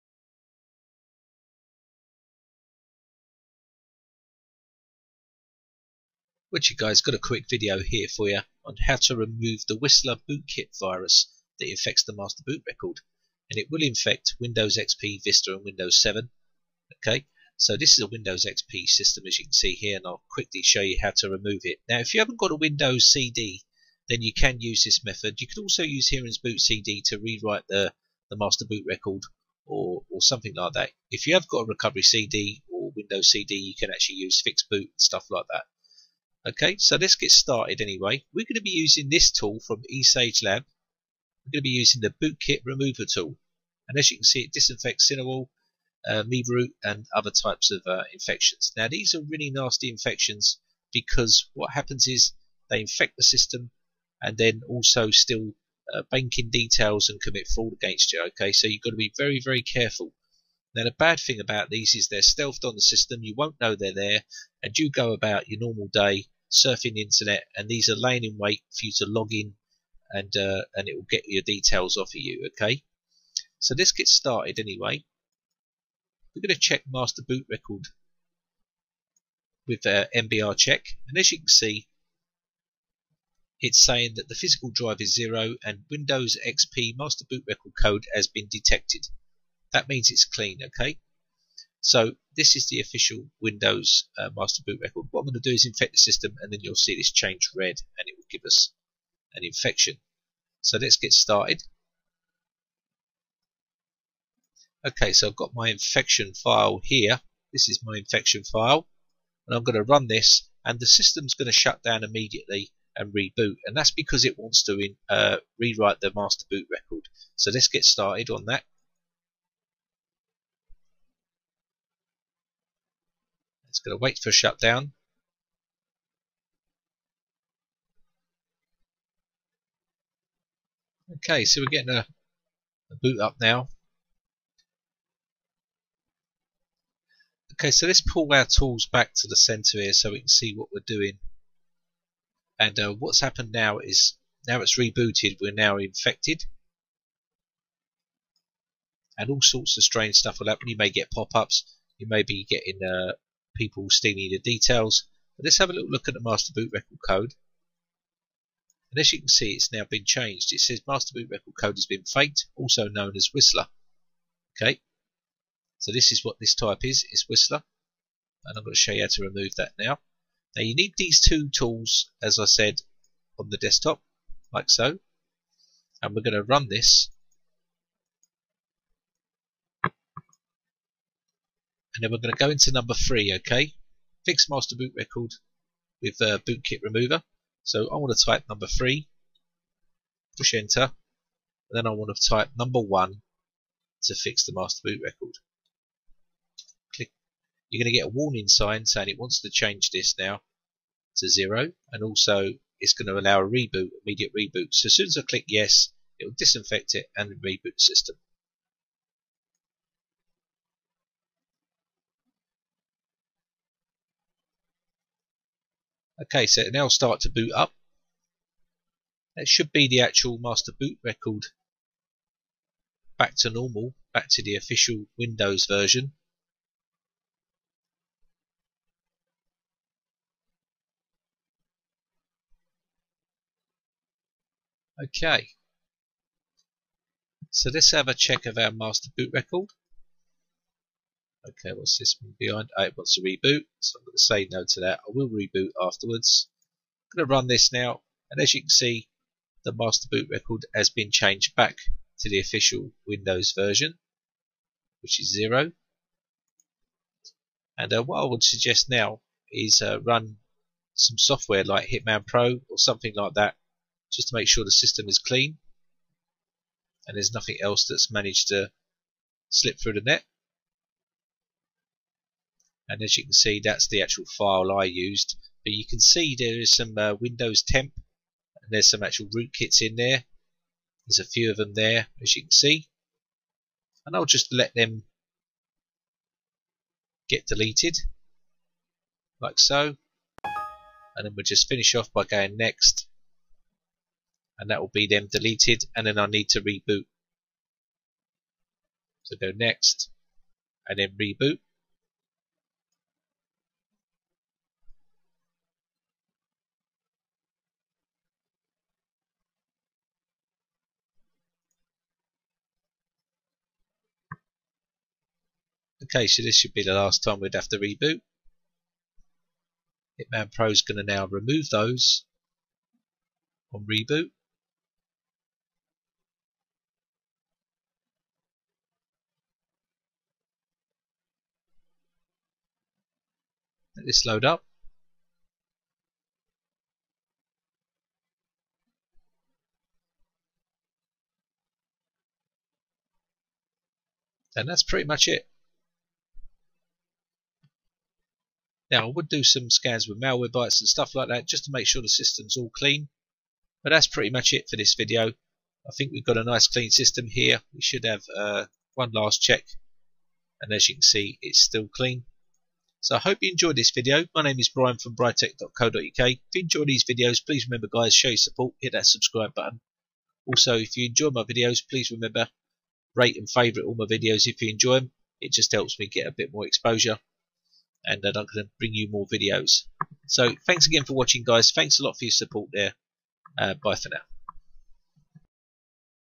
Remove Whisler, Mebroot, Sinowal Bootkit Virus Phishing Trojan by Britec
Infected System
Mebroot will install Torpig as payload and Torpig is by far the nastiest thing we have ever seen. Generally, it:
* will steal login and other personal or confidential details from banking websites
* can inject any HTML content into any website (websites can be encrypted with or without EV-SSL.) without detection
* can capture CAPCHA and compromize virtual keyboards
* can use the information in real-time to defeat One-Time-Passwords
* has configuration files for many banking sites so that it knows exactly what to look out for
* is incredibly hard to detect
* works system-wide and therefore any browser is affected. (Yes, you heard right. Firefox and Chrome users are also affected)

So how does it work?
Well, we are still reverse-engineering and analyzing the trojan in detail, however after infecting the Master-Boot-Record, it employs a complicated mechanism to injects itself into the ATAPI Harddrive Driver to then inject core windows components (svchost.exe and services.exe) which then will hook/redirect functions for all processes that are used for internet transmissions. What's important is that your webbrowser (Internet Explorer, Firefox, Opera, Chrome, ...) is infected and they don't even know it!

So what does Mebroot/MBR/Torpig do?

As said before, it is after your login credentials and personal information and the ability to manipulate this data either in real-time or use at a later date. It will either simply steal your data directly as it is typed or inject HTML code into the banking website to gather additional information.

1) Steal authentication data (including defeating virtual keyboards)

The stolen data is stored locally in a file (c:\windows\temp\rg4sfay in our case) and will then transfer this file to the malicious hosts.

2) Inject HTML Code into the banking website to steal additional data. banking services where additional information is requested. However as these forms appear after the customer logged in and come from an apparent trusted site, the success rates for the perpetrators of this trojan are much higher and more effective than ever before.

this Trojan injects itsself into various kernel drivers (atapi.sys in this case) . However this injection is only done in memory and no malicious components are ever written to the harddrive. This is why detection from Antivirus Engines is so low.

However as Torpig wants to steal data from your web browser process, it will hook key functions of the webbrowser process by patching the Import Address Table (IAT).